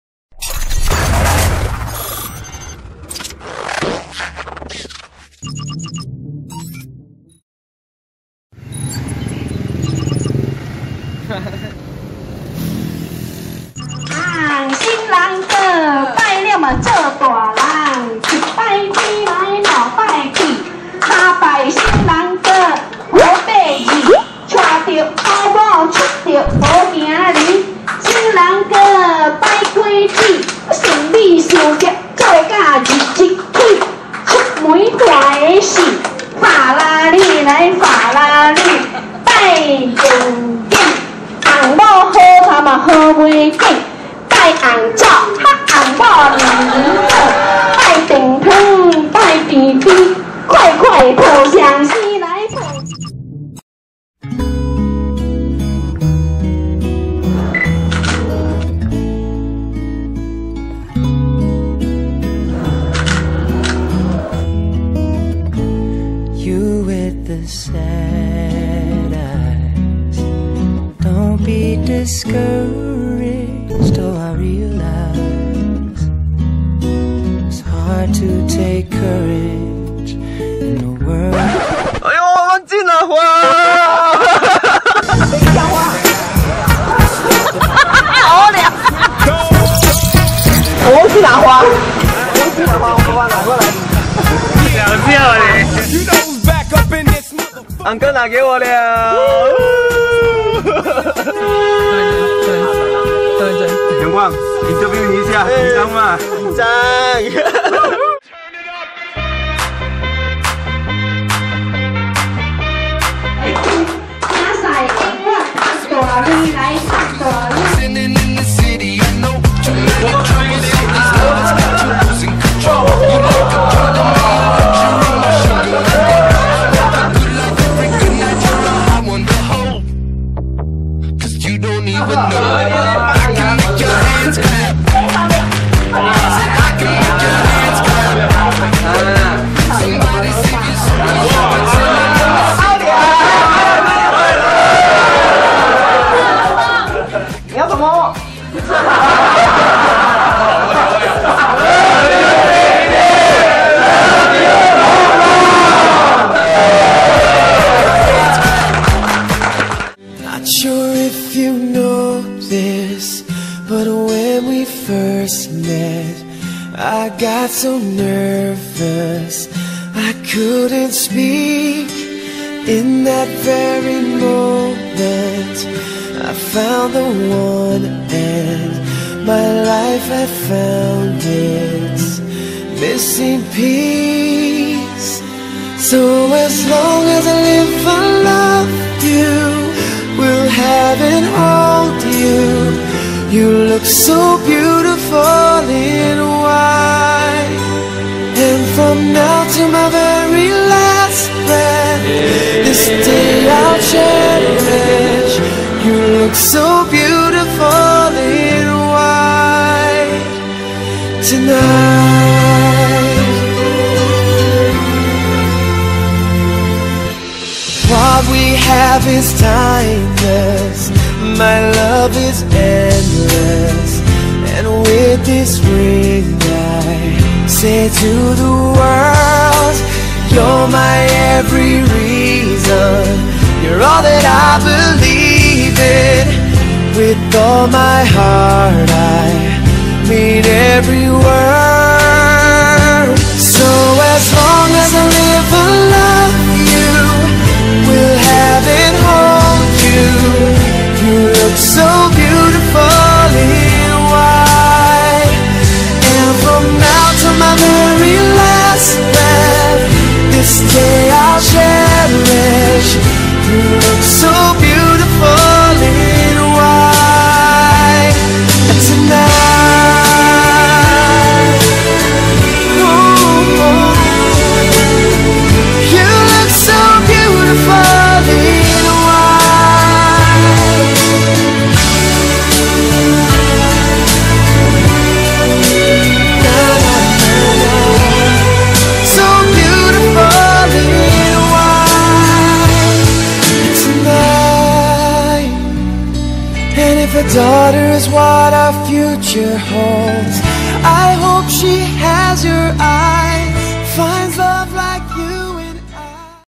YAN KUANG & FONG ZYIN Wedding (15-09-2018)
The Sweetest Moment for Yan Kuang & Fong Zyin ! !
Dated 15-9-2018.